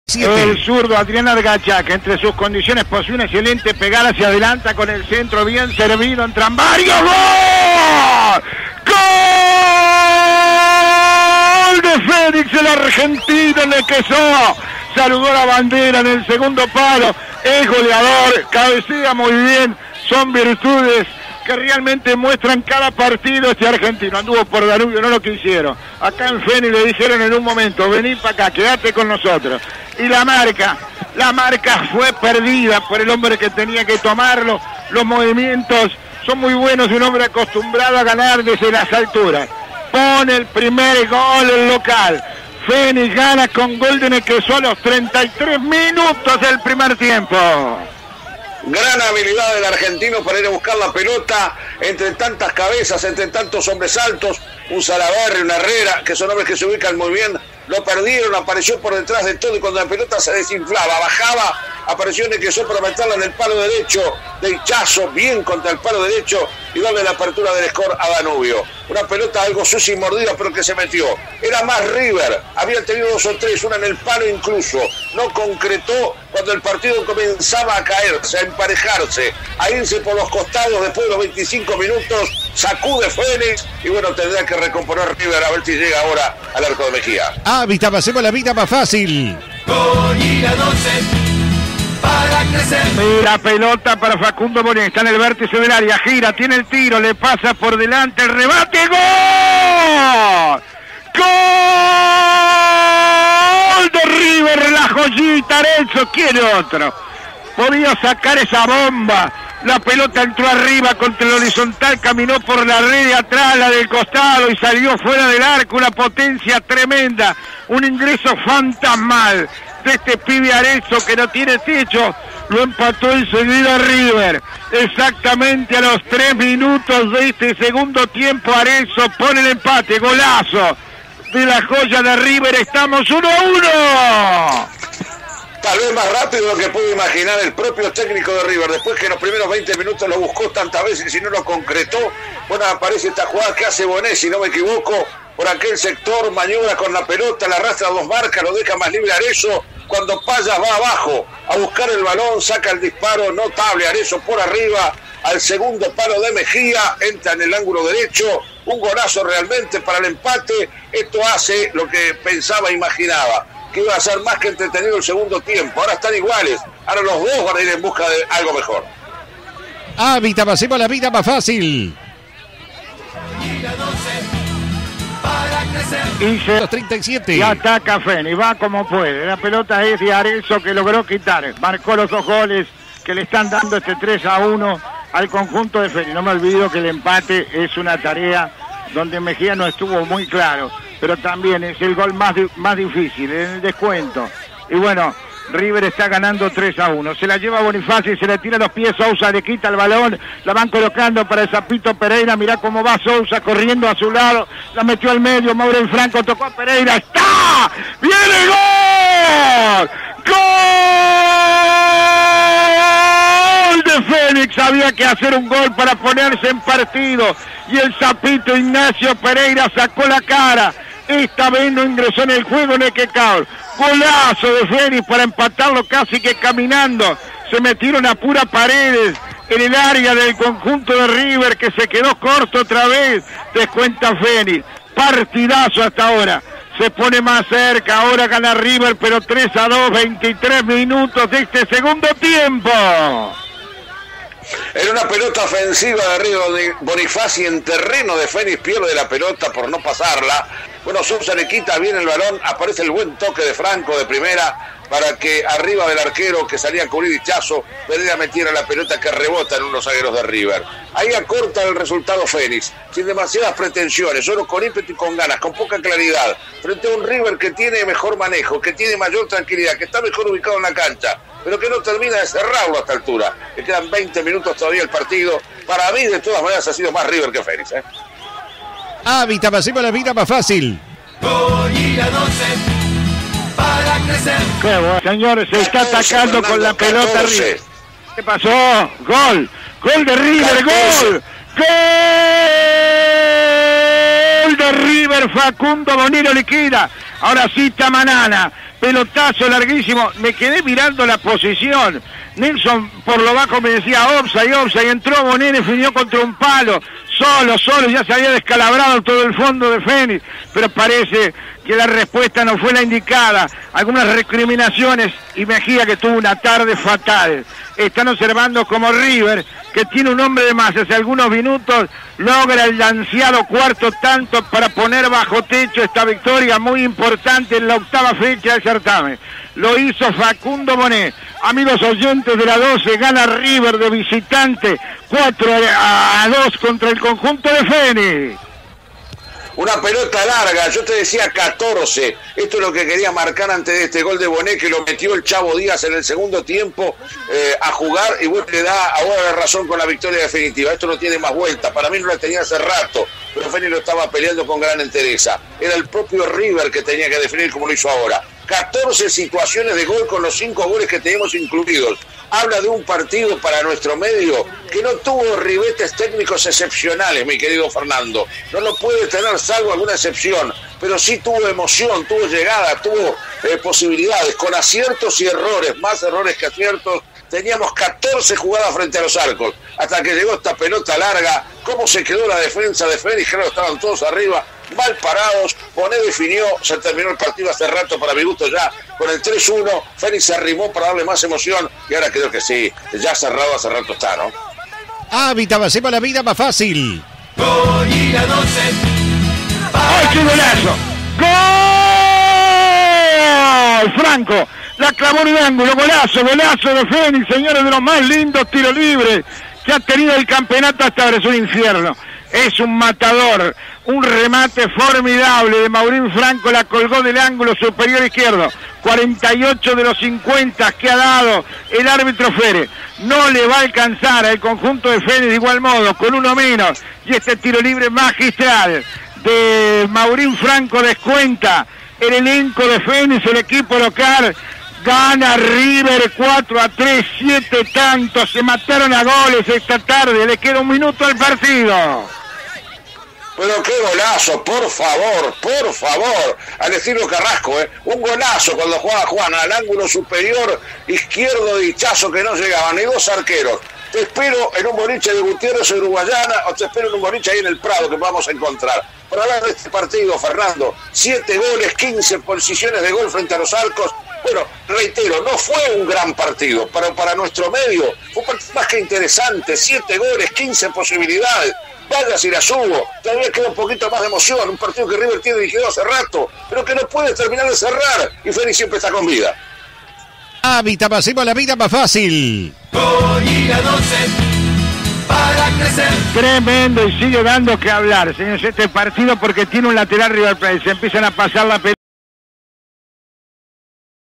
Fenix 3 River Plate 4 Apertura 2021 (por Fernando Martínez Casella)
Los goles del partido por Fernando Martínez Casella y Nelson Foliatti para CX 12 Radio Oriental 770 AM.